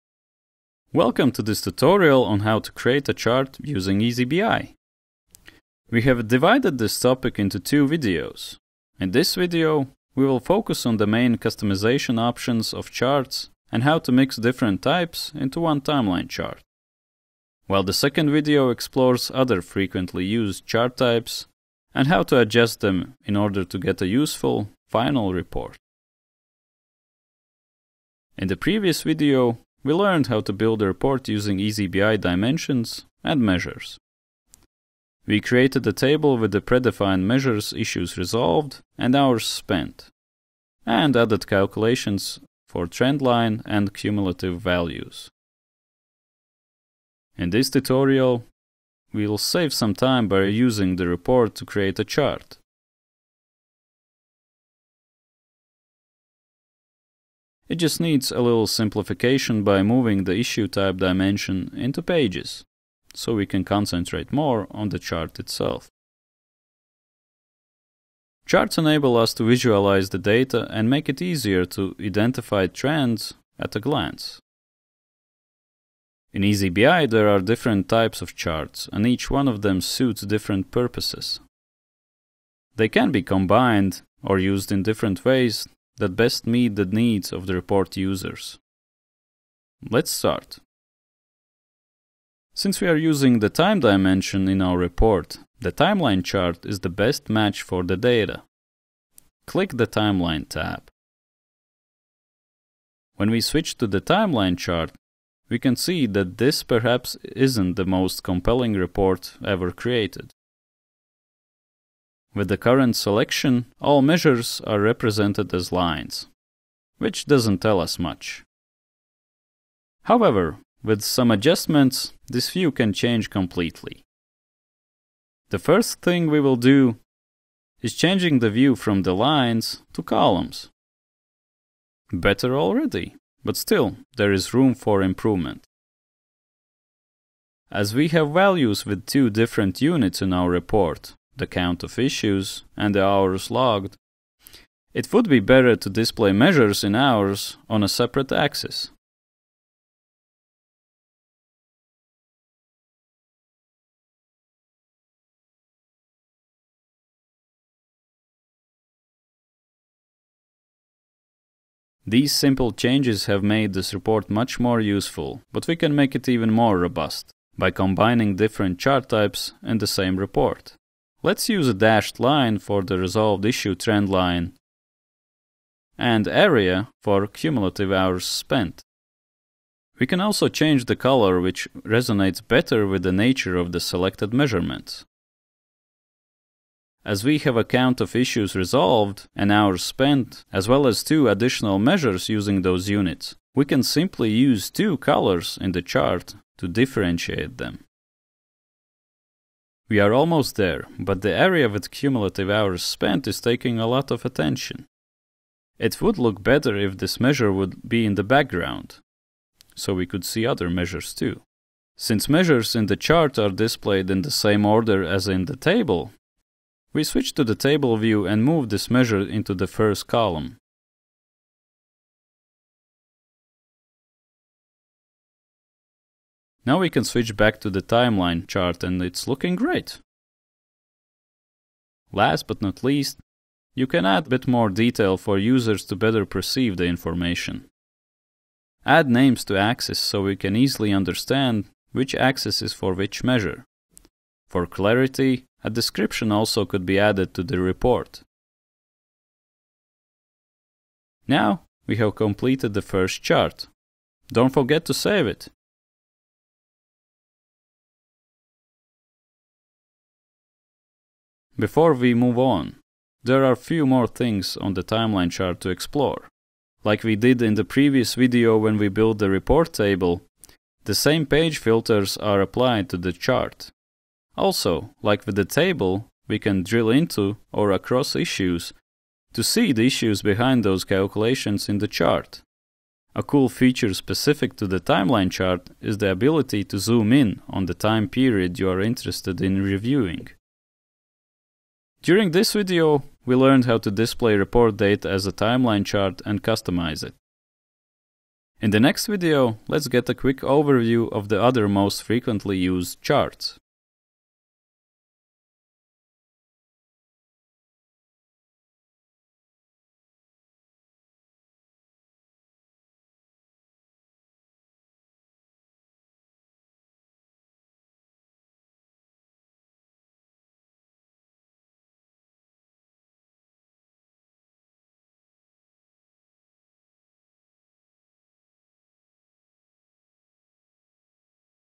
Use charts in eazyBI (Part I)
Displaying report data in charts is essential to perceive the message. Start with a timeline chart and get to know more options on how to customize it.